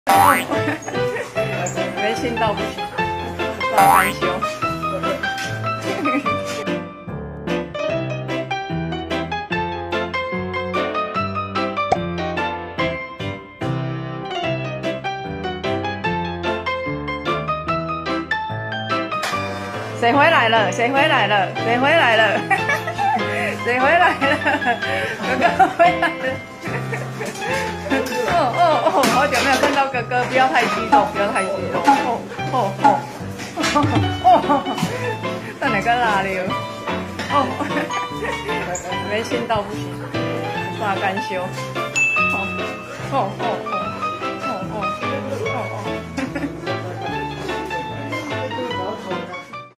看見在外讀書的小哥哥回家 2汪「爭寵式」狂親：豪想你～
影片授權：網友 林芳吟